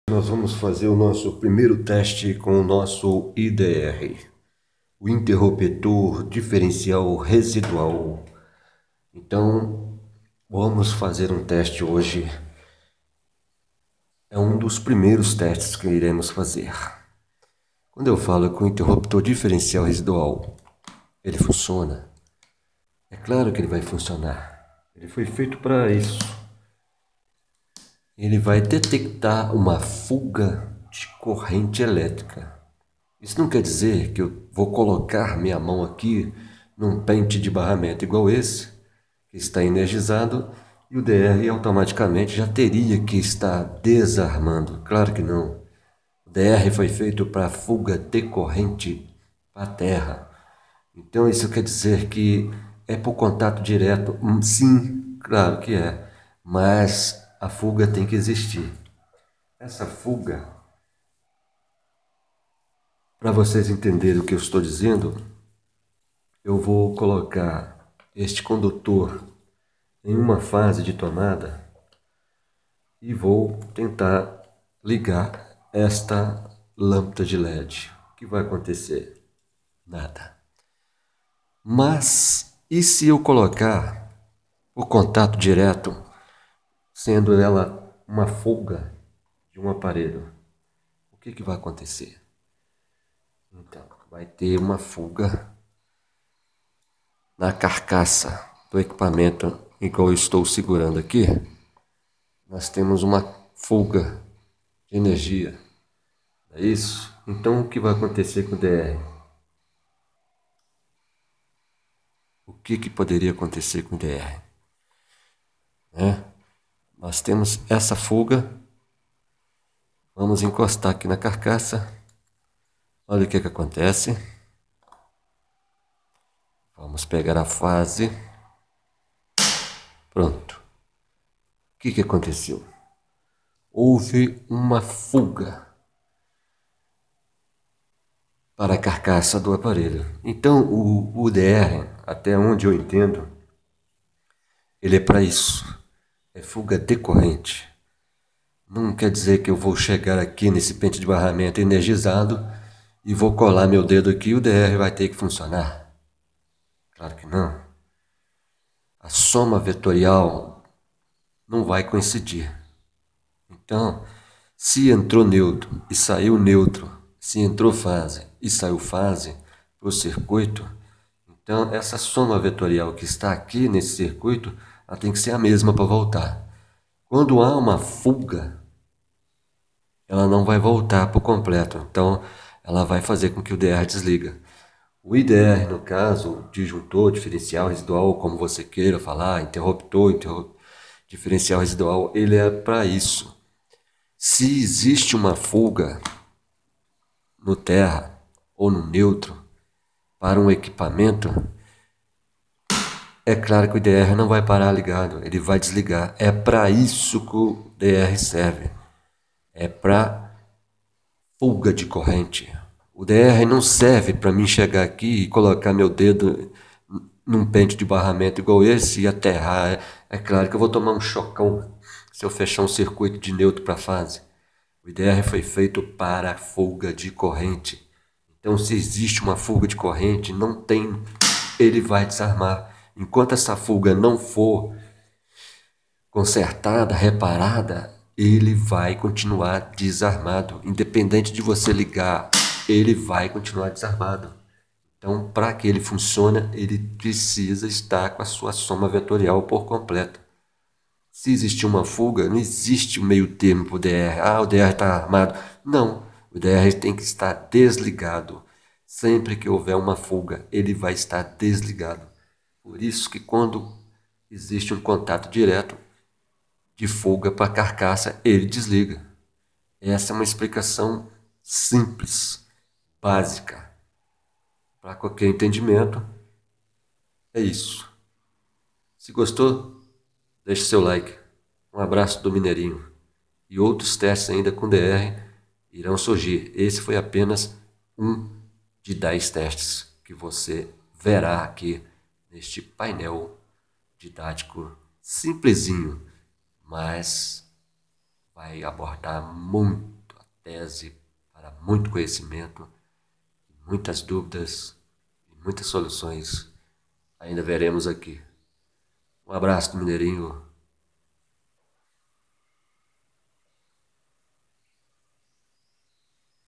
Testes com DR - O primeiro de 10 testes....!Fuga !
Inicia-se hoje,02/10/2019 os questionamentos e testes com o Interruptor Diferencial Residual ,popularmente conhecido como DR.Neste primeiro teste,acionamento por fuga para "carcaça" .